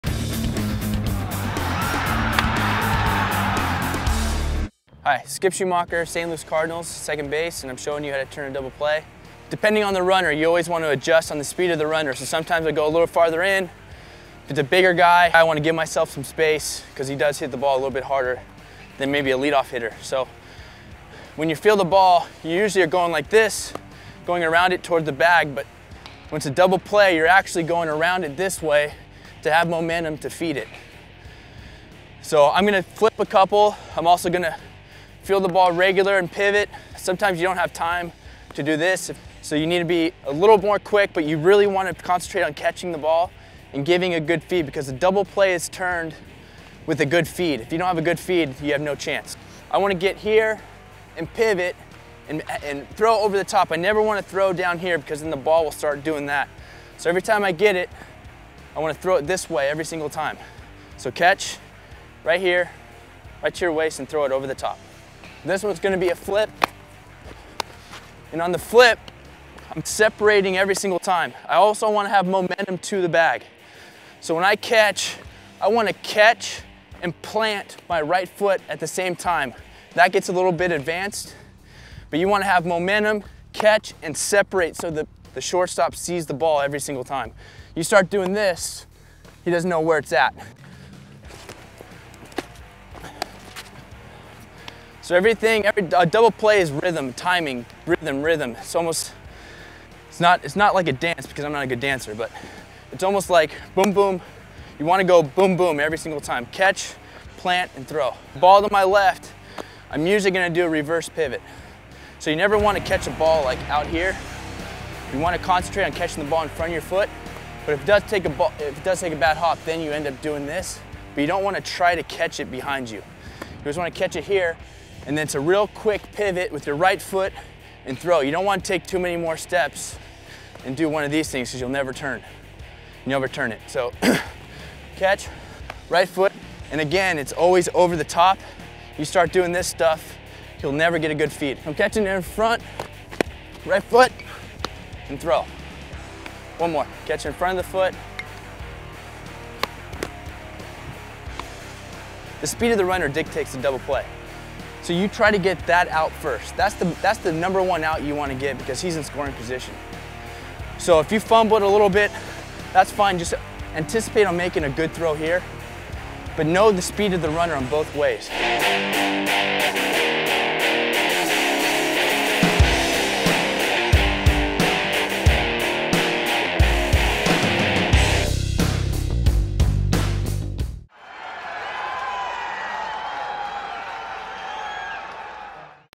Baseball Skills: Skip Schumaker Fielding Double Play Balls
Skip Schumaker, second baseman for the St. Louis Cardinals, provides instruction for fielding a grounder and delivering the feed to turn a double play.